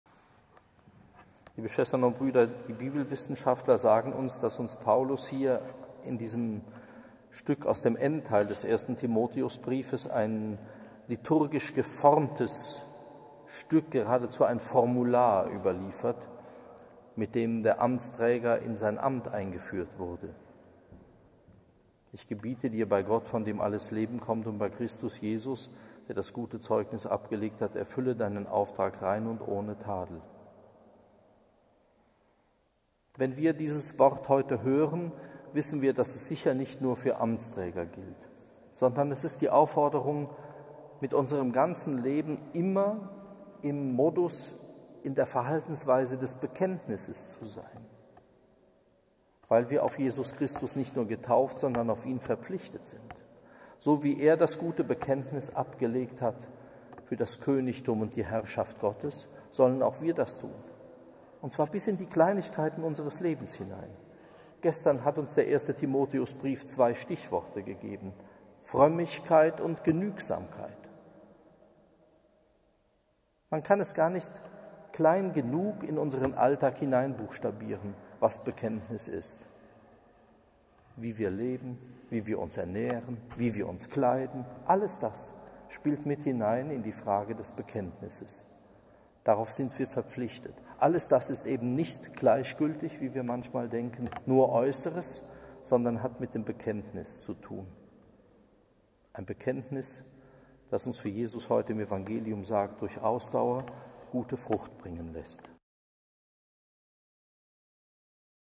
Was der Modus unseres Lebens sein sollte
Rektor Dr. Johannes J. Kreier am Samstag der 24.Woche im Jahreskreis im Kloster am Rande der Stadt/ SB.